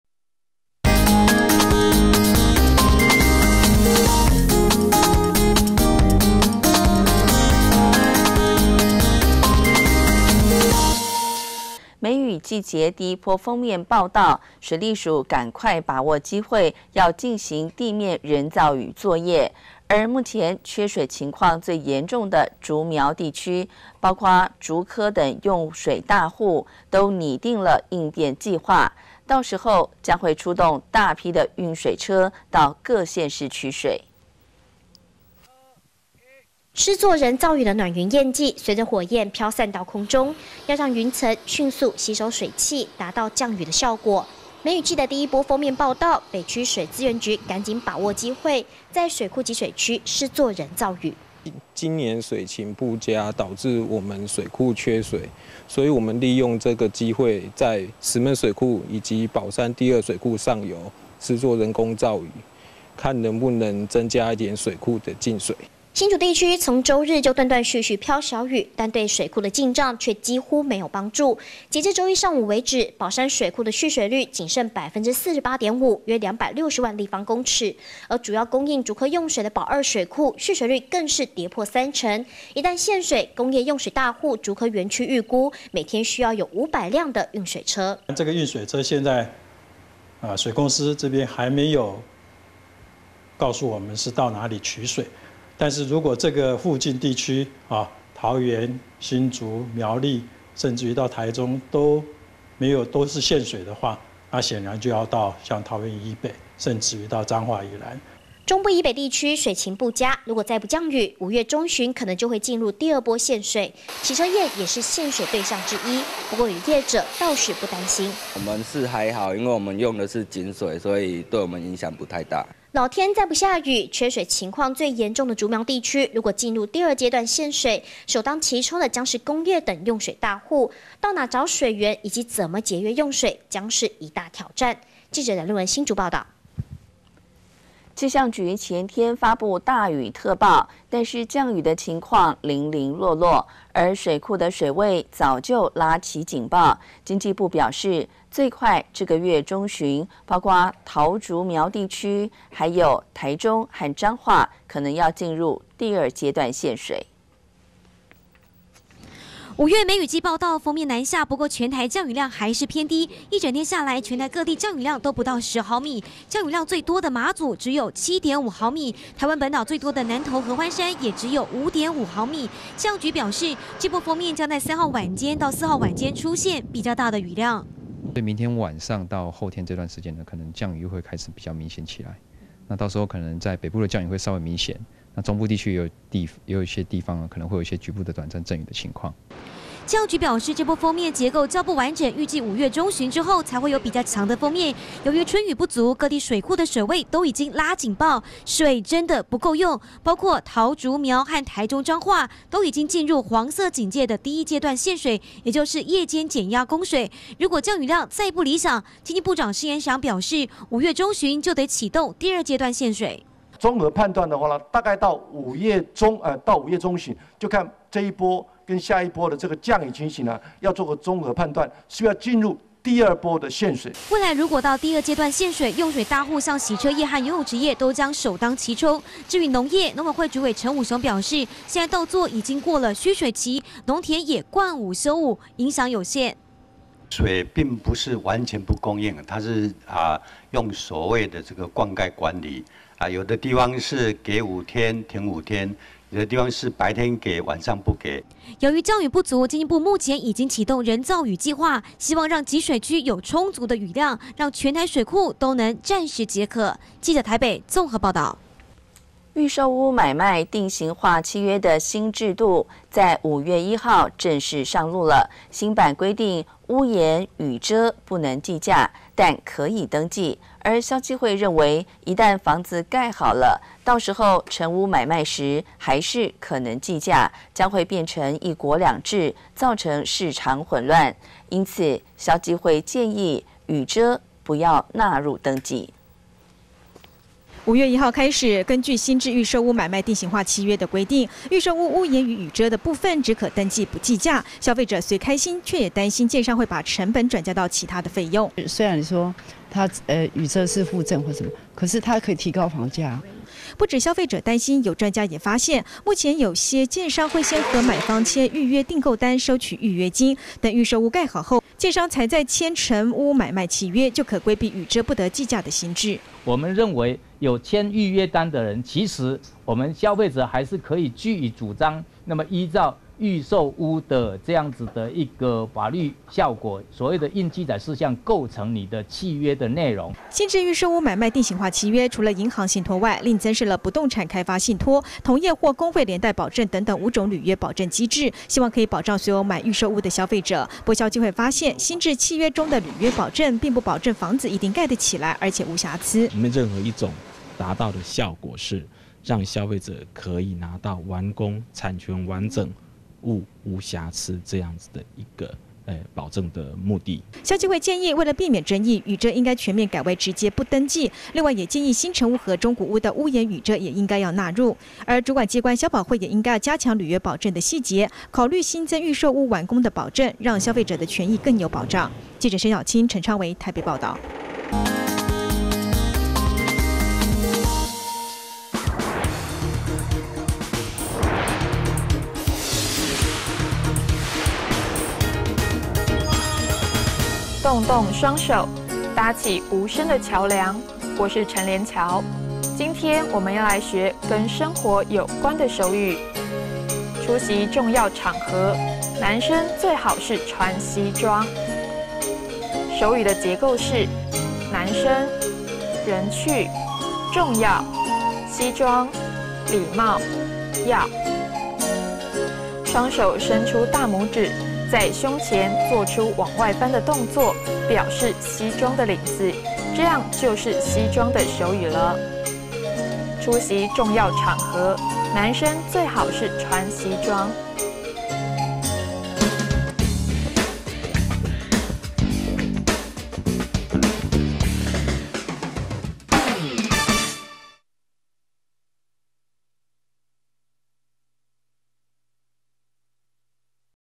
2011-05-03 公視手語新聞
紓解北台灣缺水 利用鋒面人造雨
若雨少 局部區中旬第二階段限水
消基會:防計價漏洞 雨遮應不登記